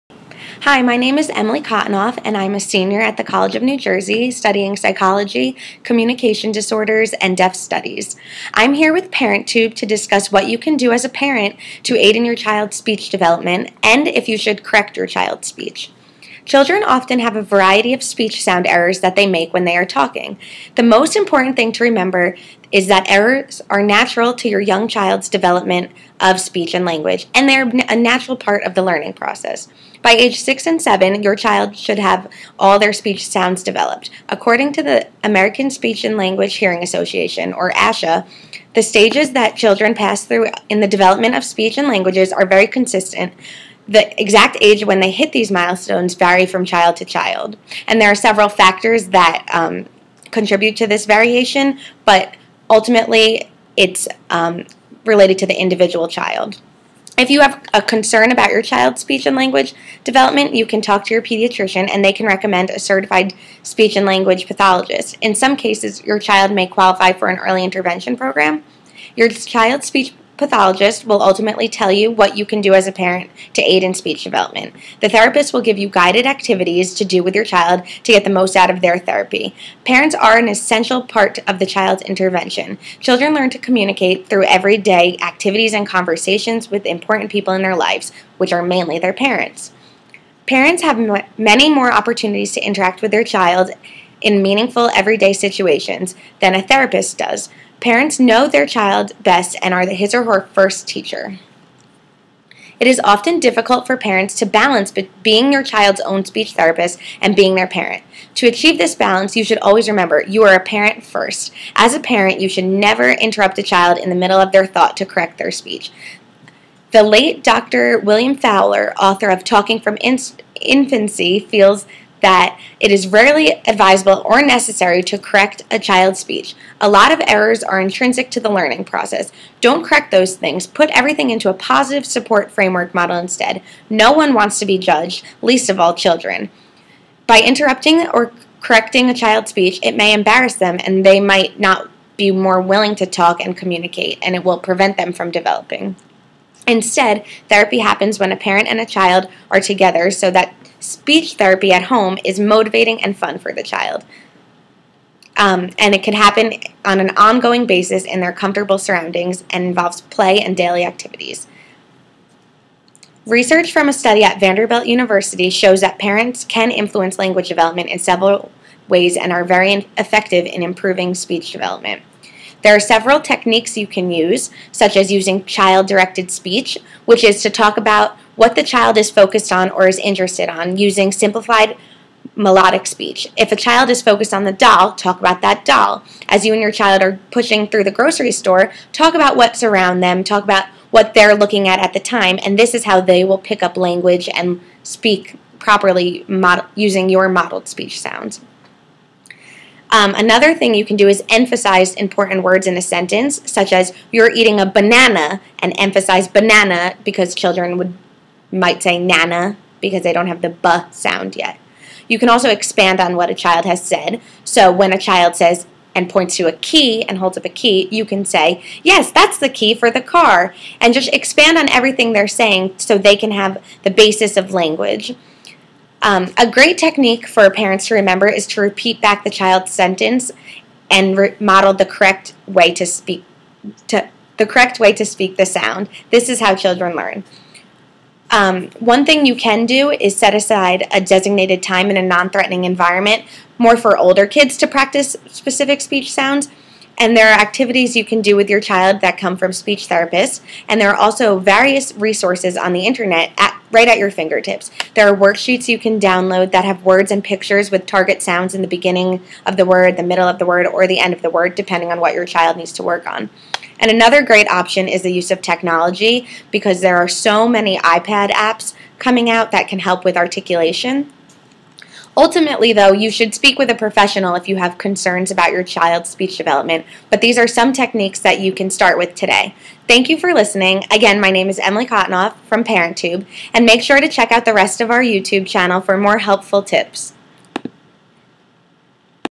Should you correct your child's speech?
What you can do as a parent to aid in your child's speech development and if you should correct your child's speech.